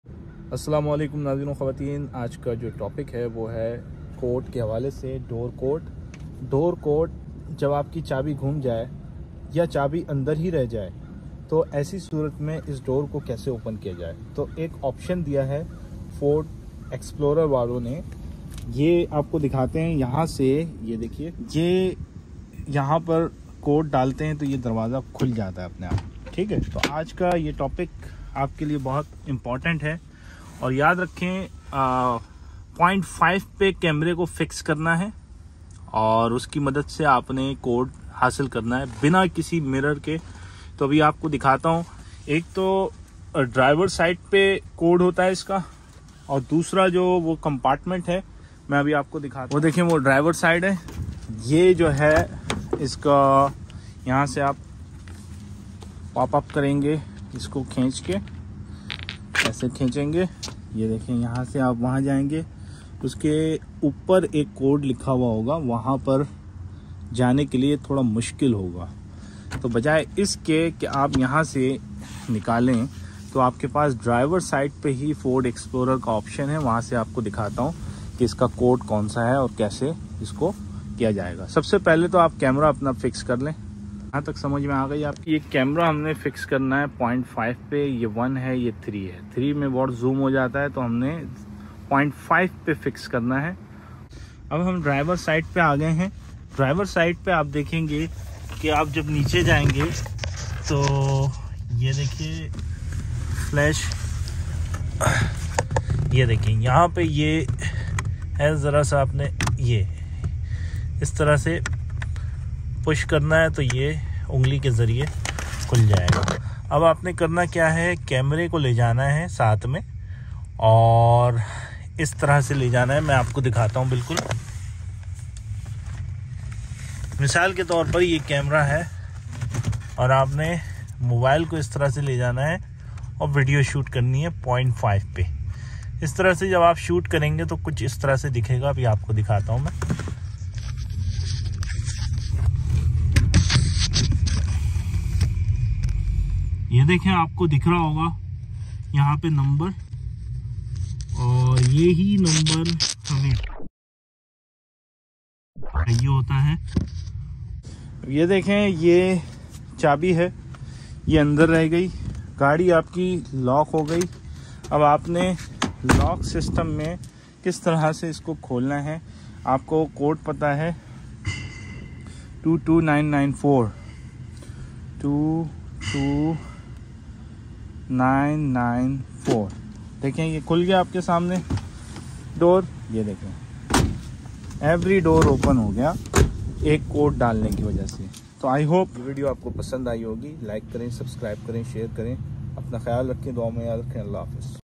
How to Find Factory keyless Entry Code On 2003-2011 Ford Explorer
Subtitle in English